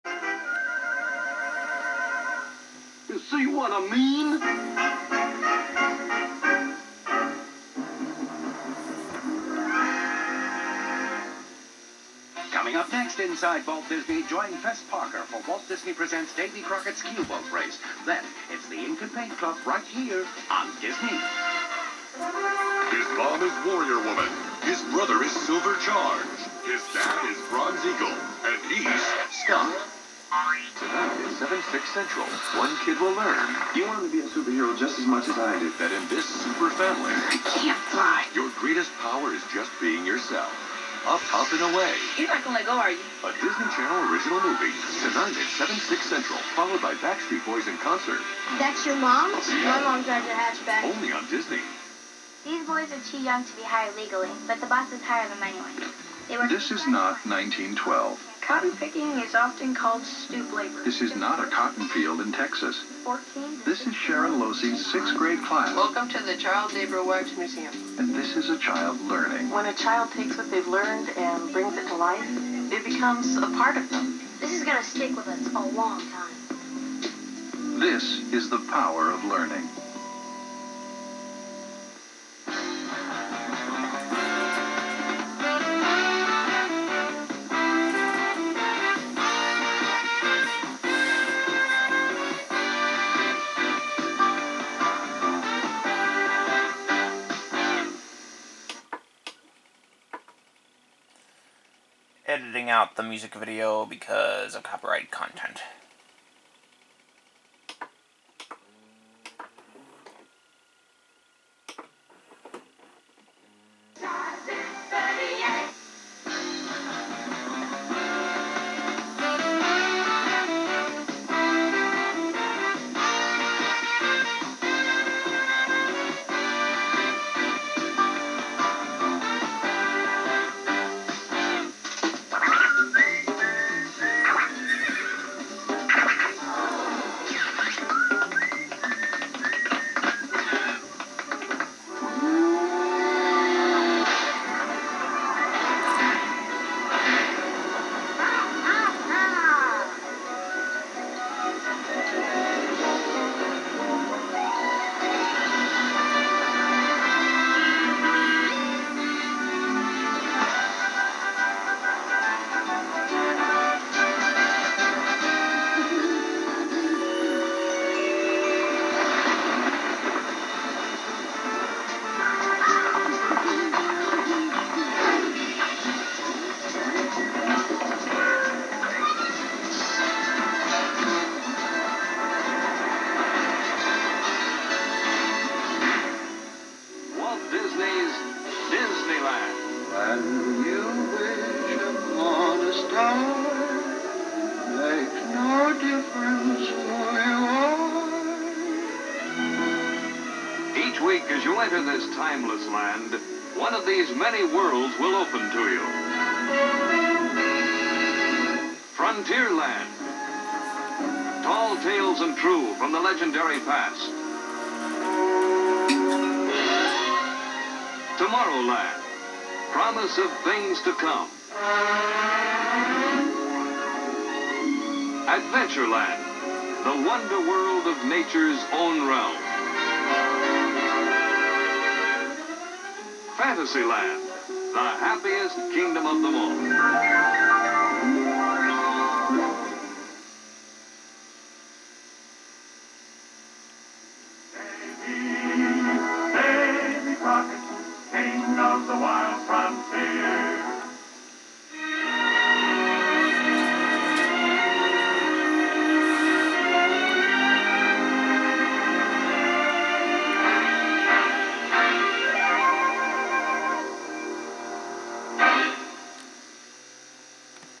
(January 22, 2000) Disney Channel Commercials: Part 3/Finale
Here is the final part! This is only a short set i'm sorry to say, but more will come in the future. This set aired after Goofy's Freewayphobia